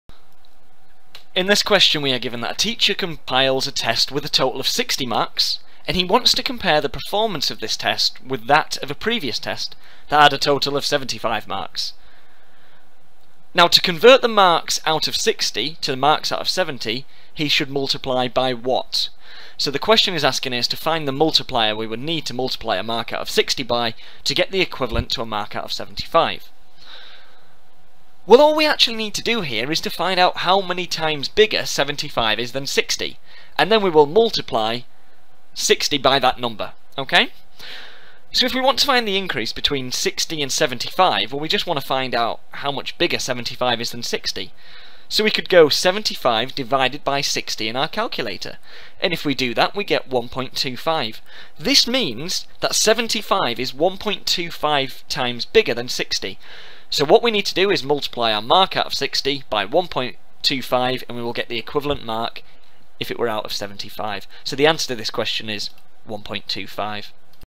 Test 4 Q28 –Numeracy Skills Government Test Model Solutions
Numeracy professional skills government practice test model solutions test 4

Model solutions for numeracy professional skills government practice test. This includes numeracy skills test tips and both the numeracy skills test mental sections and numeracy skills test written sections.

for numeracy skills tuition and numeracy skills tests.